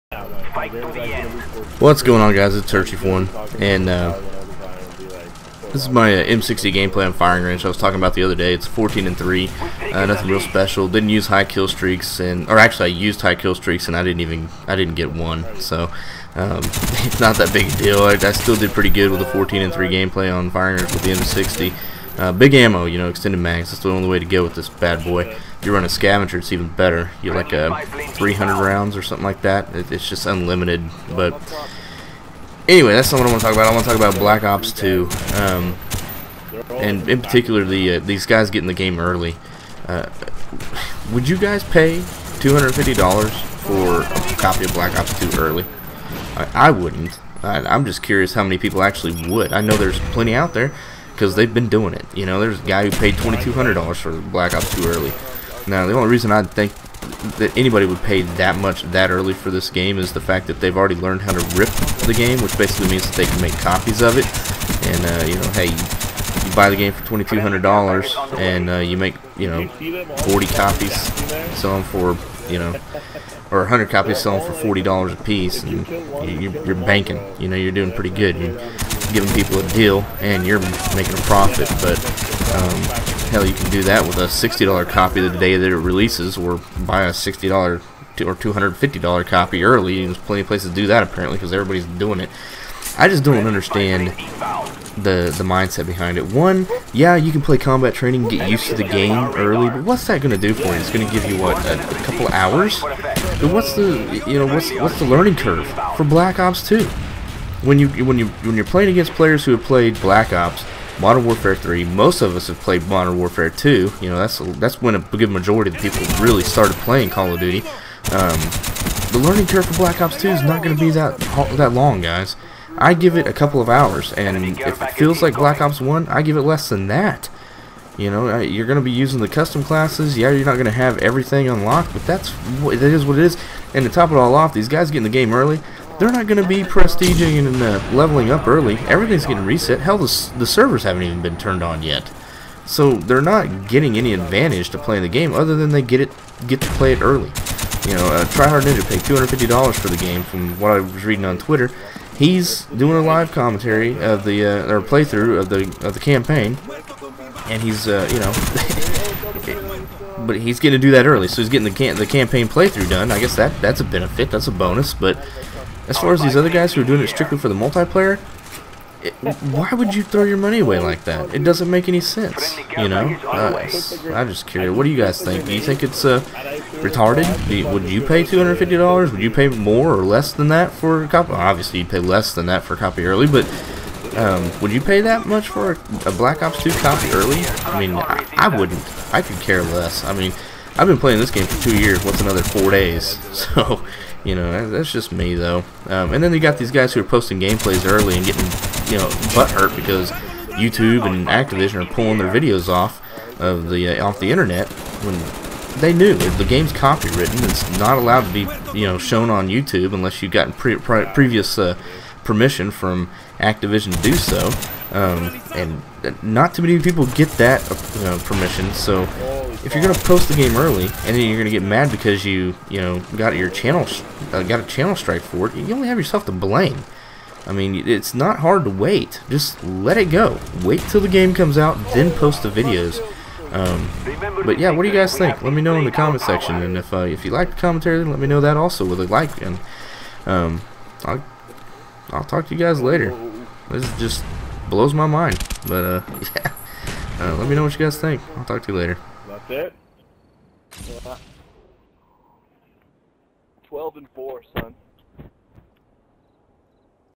Paying 200+ for BO2 early, are you dumb?!
I would love to have the game early but I'm near dumb enough to pay over 100 dollars for it just so I can play combat training for 4-5 days before the servers get turned on.